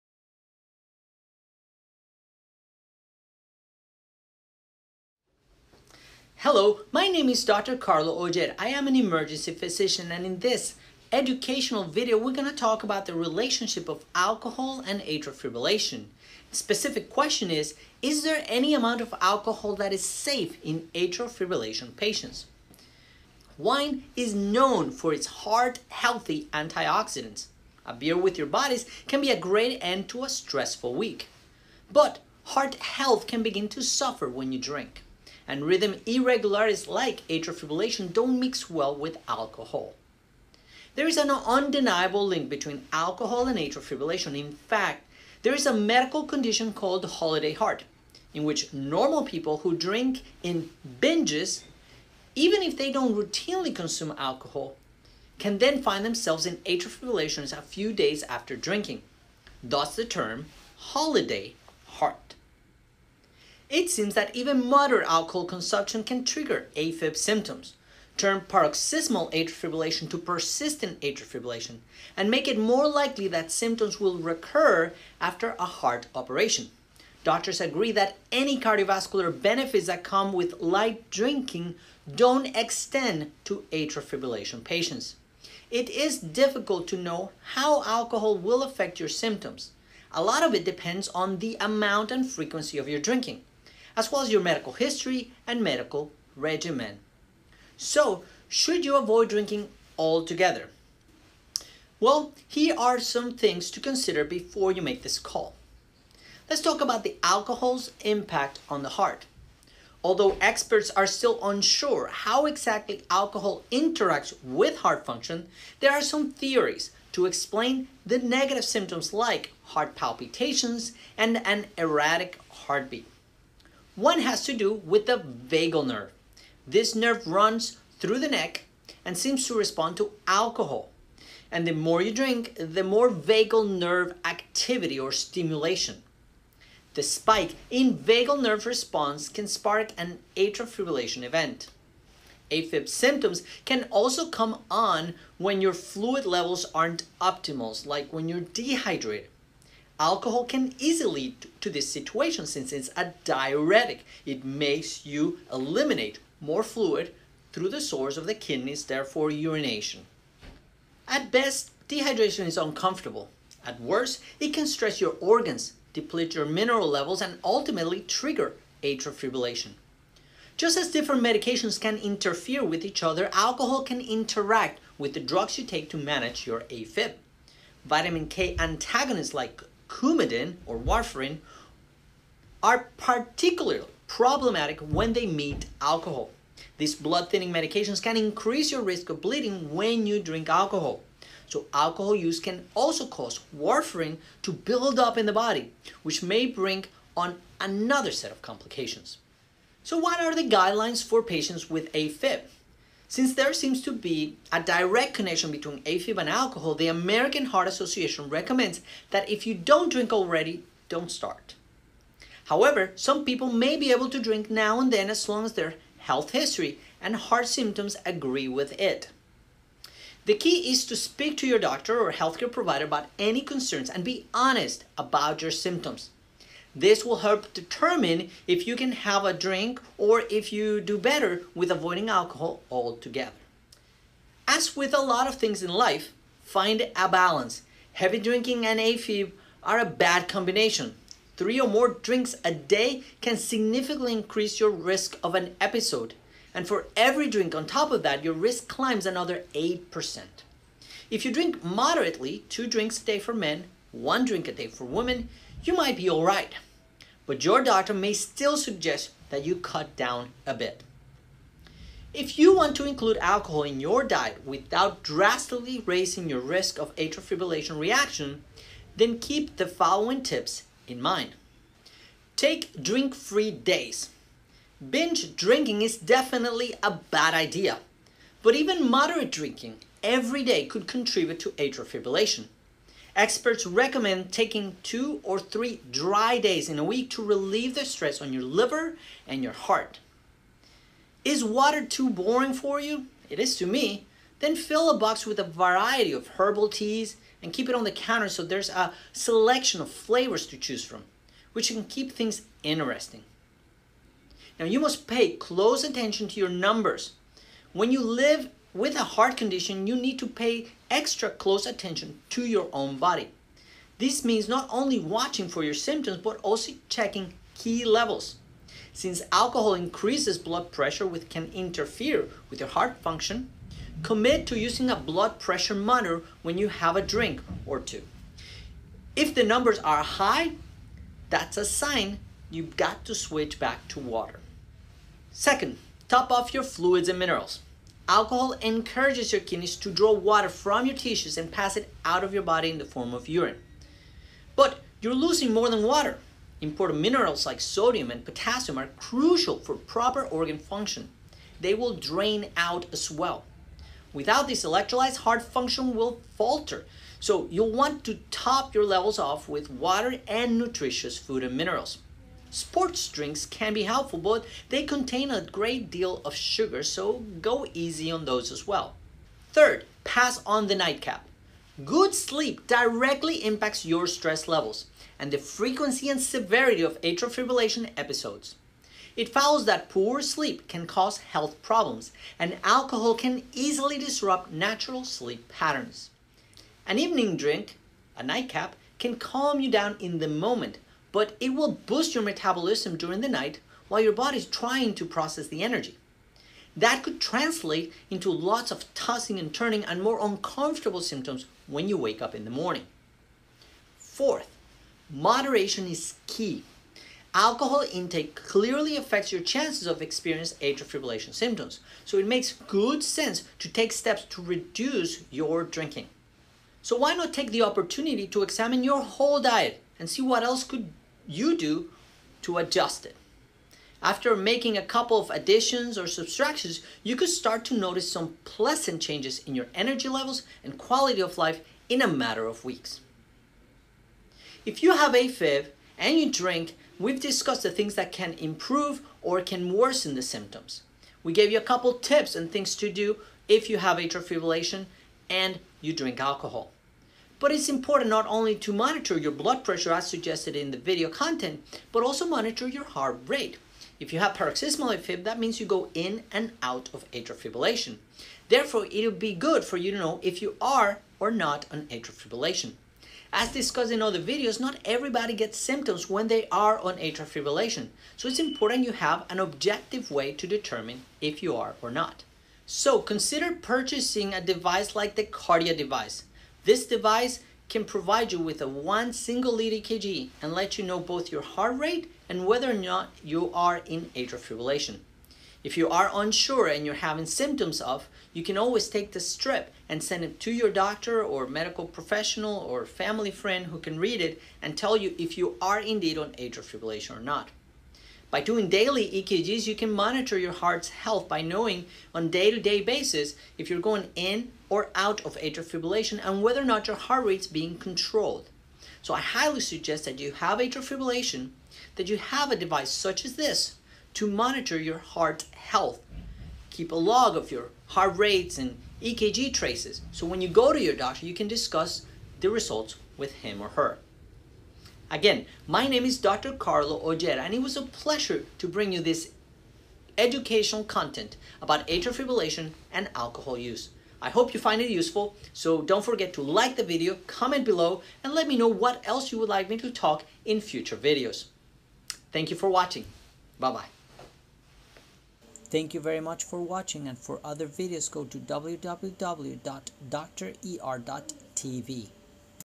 alcohol and atrial fibrillation, by Dr. Carlo Oller
Is any amount of alcohol safe for AFib patients?

Wine is known for its heart-healthy antioxidants, a beer with buddies can be a great end to a stressful week, and cocktails will liven up a party. But heart health can begin to suffer when you drink, and rhythm irregularities like AFib generally don’t mix well with alcohol.

There is an undeniable link between alcohol and atrial fibrillation. In fact, there is a medical condition called Holiday Heart, in which normal people who drink in a ‘bing’ even if they do not routinely consume alcohol can then find themselves in Afib few days after drinking. Thus the term HOLIDAY HEART.

It seems that even moderate alcohol consumption can trigger AFib symptoms, turn paroxysmal AFib to persistent AFib, and make it more likely that symptoms will recur after a heart operation. Doctors agree that any cardiovascular benefits that come with light drinking don’t extend to AFib patients.

It’s difficult to know how alcohol will affect your symptoms – a lot depends on the amount and frequency of your drinking, as well as your medical history and medication regimen. So…Should you avoid drinking altogether? Here are some things to consider before you make that call.

Alcohol’s impact on the heart
Although experts are still unsure how exactly alcohol interacts with heart function, there are some theories to explain the negative symptoms like heart palpitations and an erratic heartbeat. One has to do with the vagal nerve: this nerve that runs through the neck seems to respond to alcohol, and the more your drink, the more vagal nerve activity. This spike in vagal nerve response can spark an AFib event.

AFib symptoms can also come on when your fluid levels aren’t optimal, and alcohol can easily lead to such a situation. Since alcohol is a diuretic, it helps your body eliminate more water, and that can leave you dehydrated. At best, dehydration is uncomfortable; at worst, it can stress your organs, deplete your mineral levels, and ultimately trigger AFib.

get your Kardia HERE, and monitor your Afib
Just as different medications can interfere with each other, alcohol can interact with the drugs you take to manage your AFib. Vitamin K antagonists like warfarin or acenocoumarol are particularly problematic when they meet alcohol: these blood-thinning medications can increase your risk of bleeding when you drink. Alcohol use can also cause warfarin to build up in the body, which may bring on another set of complications.

What are the guidelines for AFib patients?
Since there seems to be a direct connection between AFib and alcohol, the American Heart Association recommends that if you don’t drink already, don’t start. However, some people may be able to drink now and then, as long as their health history and heart symptoms agree with it. The key is to speak with your doctor about any concerns and be honest about your symptoms – this will help determine if you can have a drink or if you’d do better to avoid it altogether.

Finding a balance
Heavy drinking and AFib are a bad combination – three or more drinks a day significantly increases your risk of an episode, and for every drink on top of that, your risk climbs another 8%. If you drink moderately (two drinks a day for men, or one drink a day for women), you might be alright, but your doctor may still suggest you cut down a bit.

If you want to include alcohol in your diet without drastically raising your risk of an AFib reaction, keep these tips in mind:

Take drink-free days. Binge drinking is definitely a bad idea, but even moderate drinking every day could contribute to AFib. Experts recommend taking two or three dry days a week to relieve the stress on your liver and your heart. Is water too boring? Fill a box with a variety of herbal teas and keep it on the counter so there’s a selection of flavors to choose from, which can keep things interesting.

Pay close attention to your numbers. When you live with a heart condition, you need to pay extra close attention to your body. This means not only watching for symptoms, but also checking key levels. Since alcohol increases blood pressure, which can interfere with heart function, commit to using a blood pressure monitor when you have a drink or two. If the numbers are high, that’s a sign to switch to water.

Top up your fluids and minerals. Alcohol encourages your kidneys to draw water from your tissues and pass it out of your body. But you’re losing more than water: important minerals like sodium and potassium, crucial for proper organ function, will drain out, too. Without these electrolytes, heart function will falter, so you’ll want to top your levels up with water and nutritious food. Sports drinks can be helpful, but they can contain a good deal of sugar, so go easy.